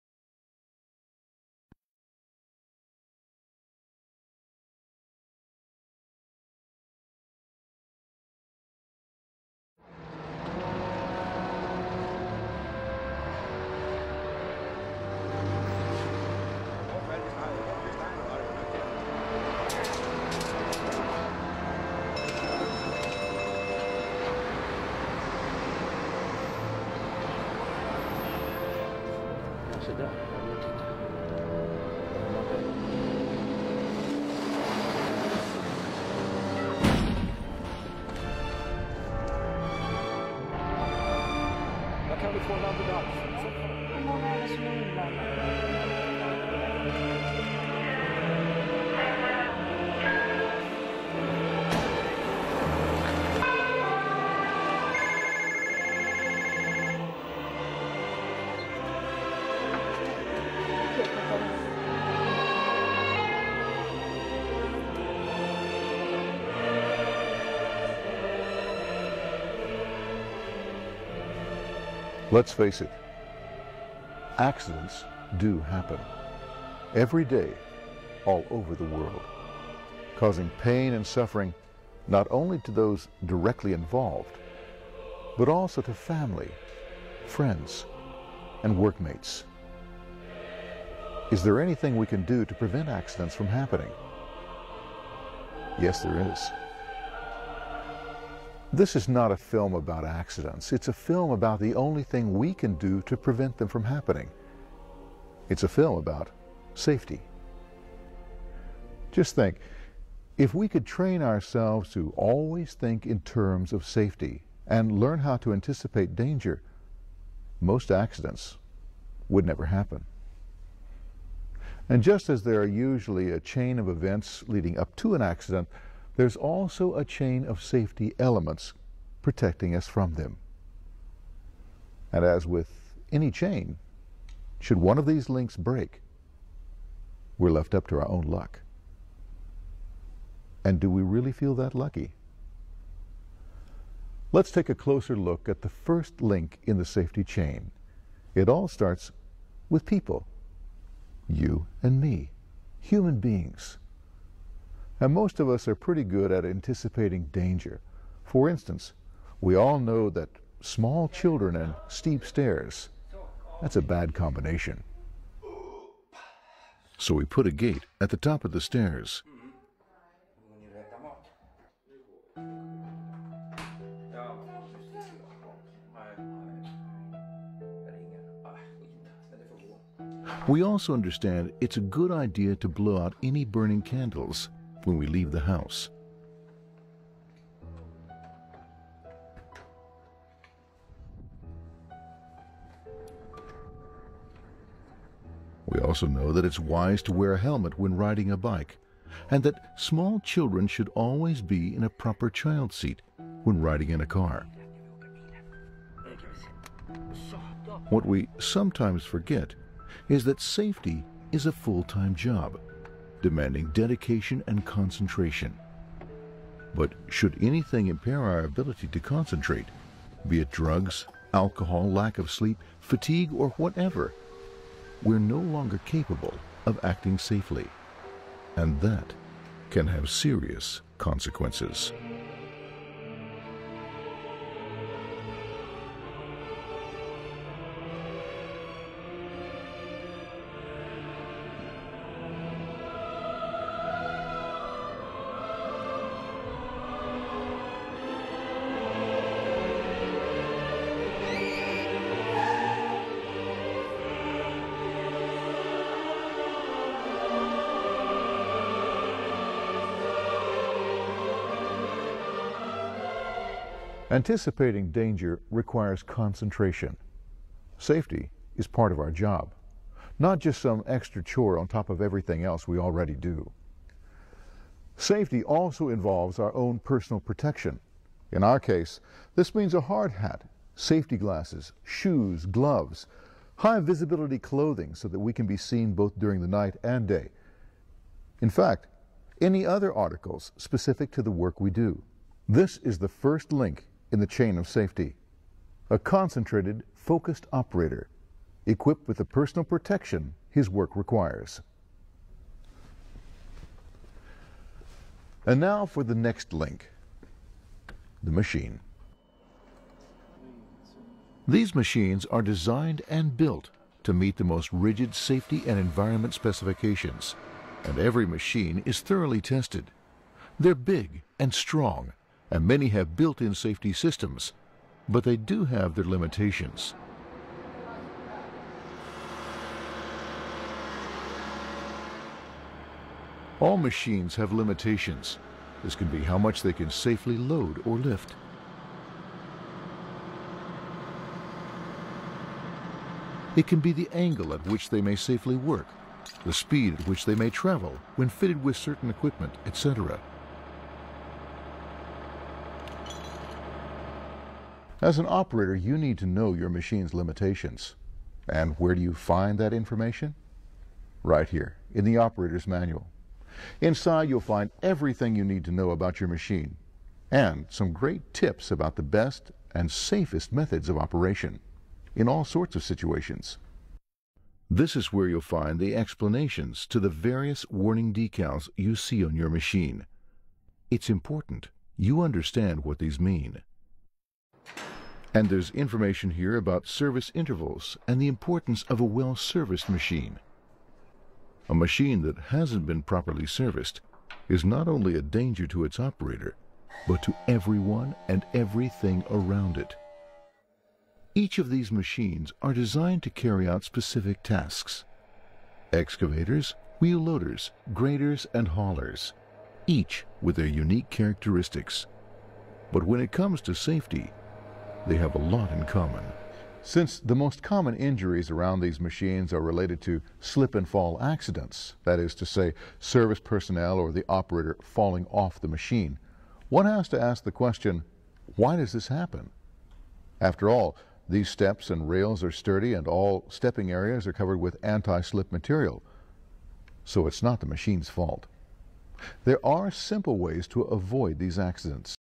16. Körteknik Volvo hjullastare: Säkerhet (safety wheel loaders) del 1
Grundläggande körteknik hjullastare/lastmaskin från Volvo Construction Equipment hos Swecon (på engelska).